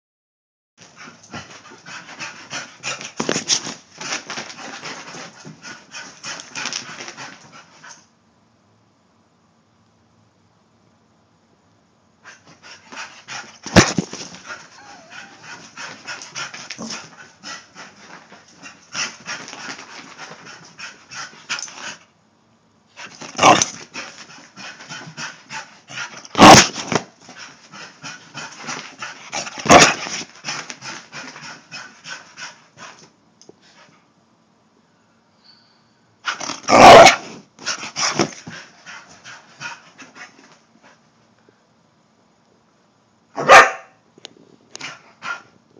Circles
My dog runs in circles lol